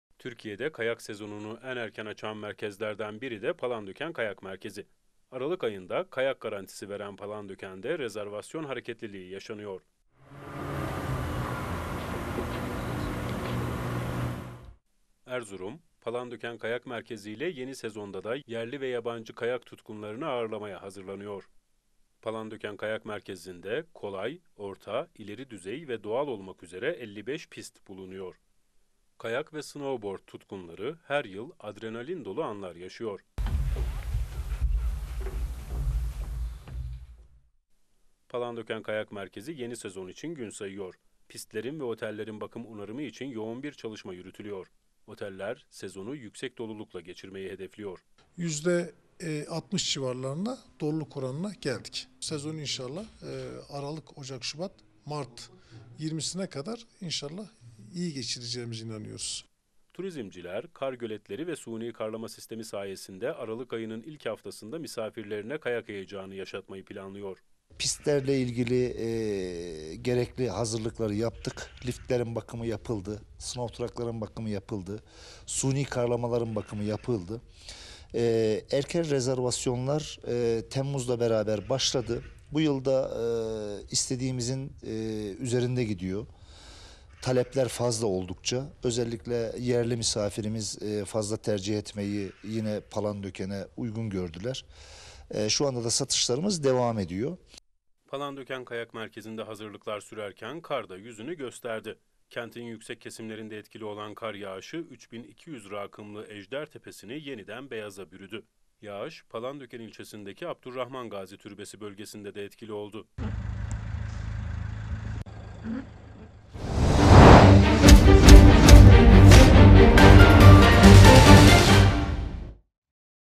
Kardelen TV / Palandöken Yeni Sezona Hazırlanıyor
Suni karlama sayesinde sezonu en erken açan kayak merkezlerinden biri olan Palandöken Kayak Merkezlerinde yeni sezon hazırlıkları başladı. Rezervasyonlar alınıyor, aralık ayının ilk haftasında kayak sezonunun açılması planlanıyor.